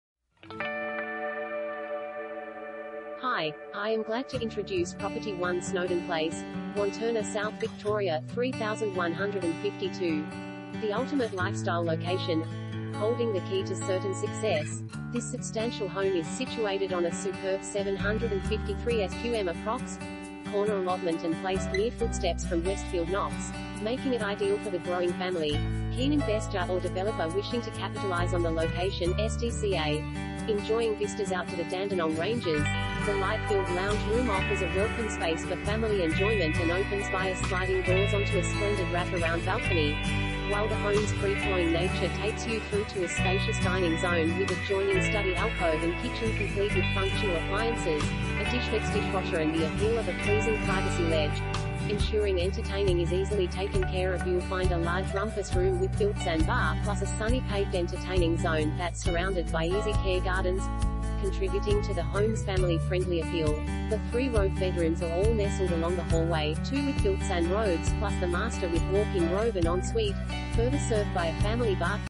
1 Snowden Place, Wantirna South VIC 3152 | AuPropertyReport.Com
1 Snowden Place, Wantirna South VIC 3152
Property Type: house
Bedrooms: 3
Bathrooms: 3
Car Space: 4
The ultimate lifestyle location.